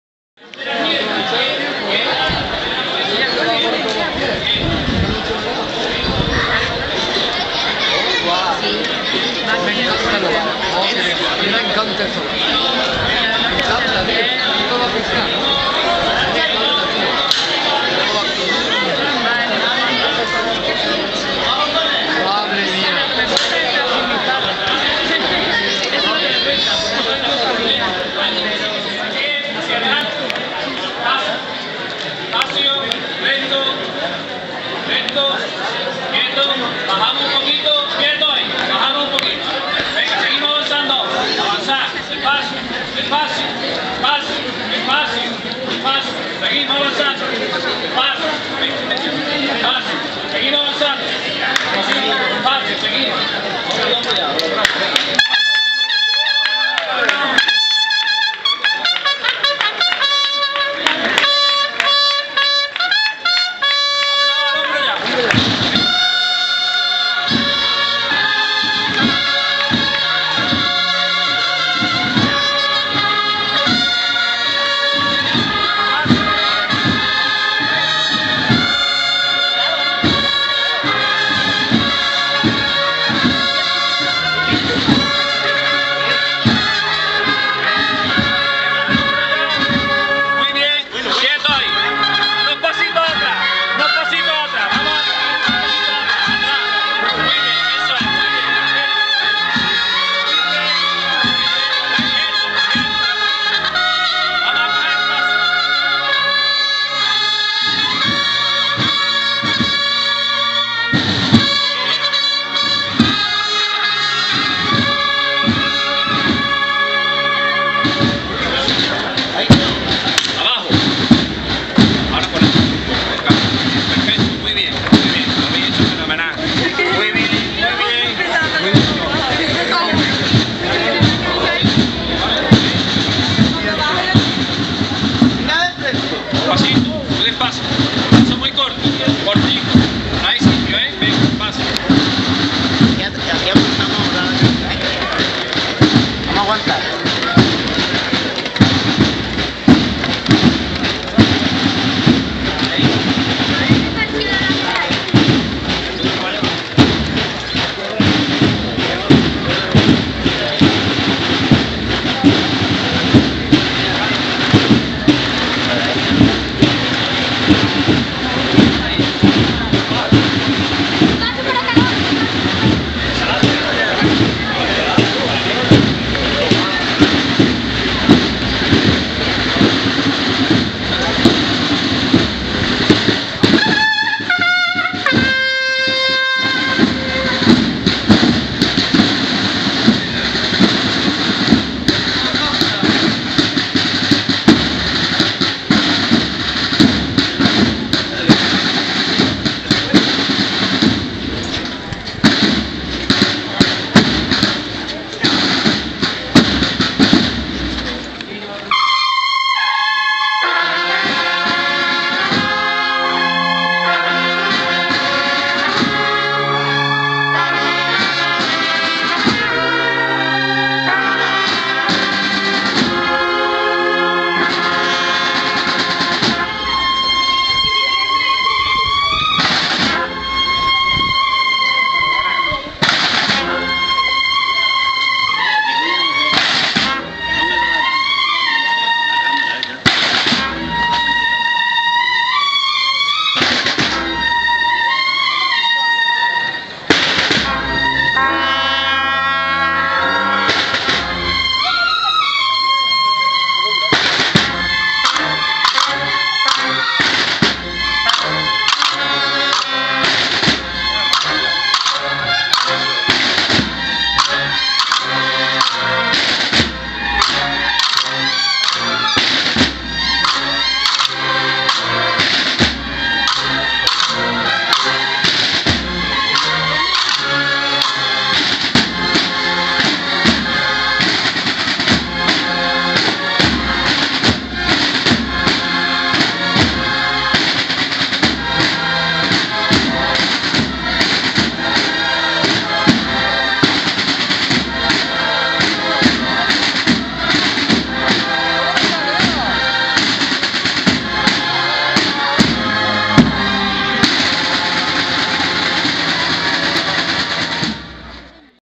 Jesús en su Entrada Triunfal en Jerusalén 2014
Salida de la Hermandad de Jesús en su Entrada Triunfal en Jerusalén, más conocida popularmente como La Borriquita, de la Iglesia-Santuario de María Auxiliadora, y en su posterior descenso por la Cuesta del Silencio, a los sones de la Banda de Cornetas y Tambores 'Preso, Soledad y Esperanza', de Montilla-Zuheros, en la Estación de Penitencia de la mañana del Domingo de Ramos, día 13 de abril de 2014.